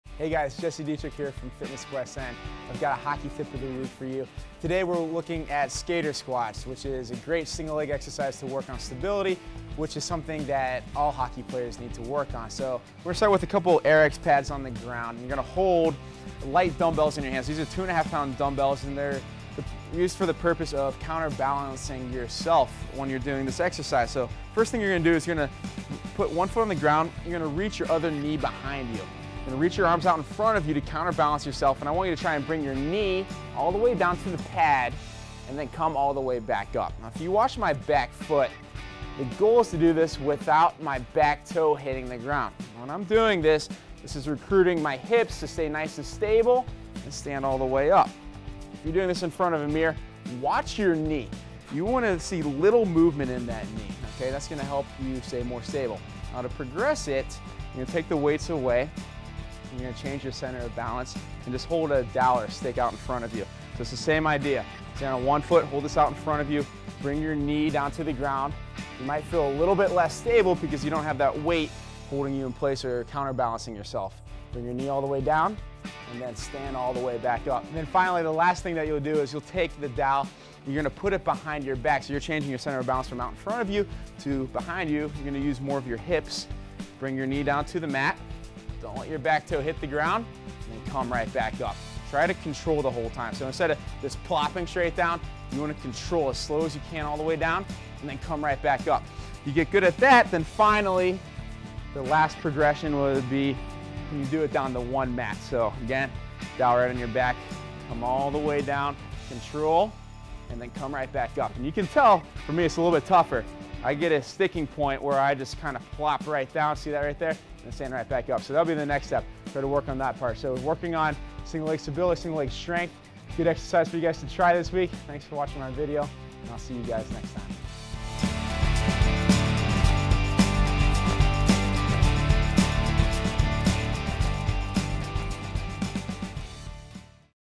Three Exercises to Improve Skating Stability (Hockey Tip)
In this video, Jesse Dietrick, CSCS, of Fitness Quest 10, goes how to progress and perform "skater squats." This is a great exercise to progress to and develop single leg balance, mobility, and stability which is very much demanded in hockey.

Perform these exercises in a well structured strength and conditioning program or before skating.